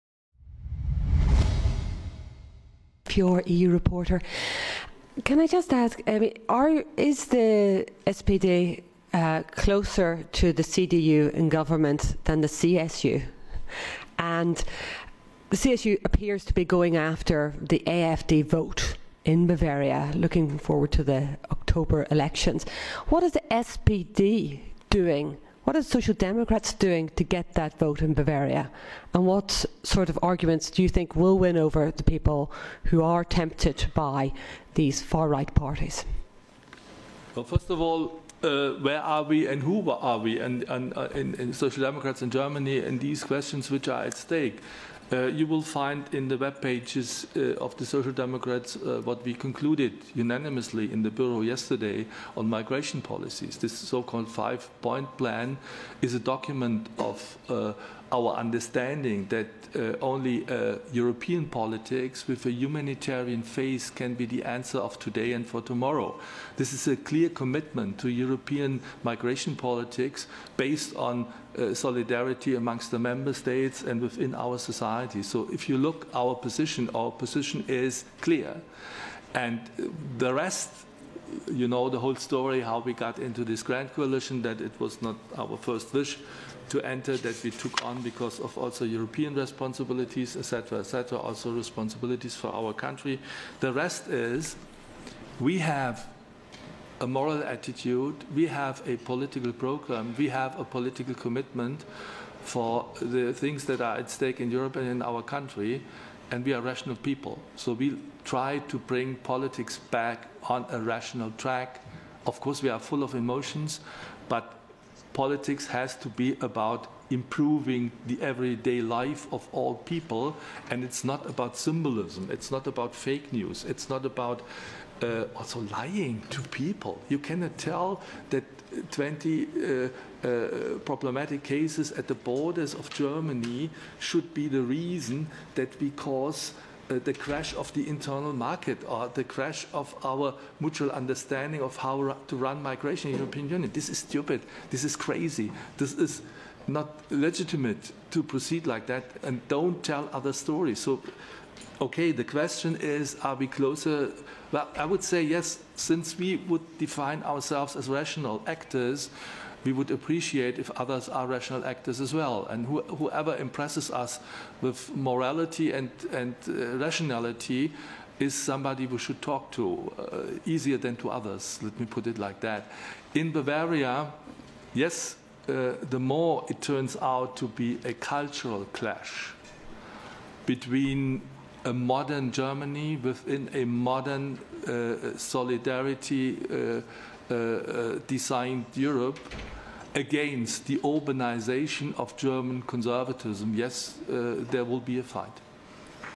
Migration: 'Politics is not about lying to people'
The German Social Democratic Party commits itself to a humanitarian and European migration policy. Asked if the SPD is closer to the CDU than the CSU, President of 'The Progressives' said that his party the German SPD would side with those parties that are with a rational and moral approach.